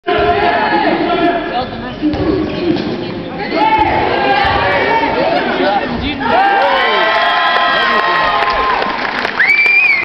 hbsm - Marseille Nord
match retour demi finale regionale gagné pour HBSM le 14/04/2013
36 - 29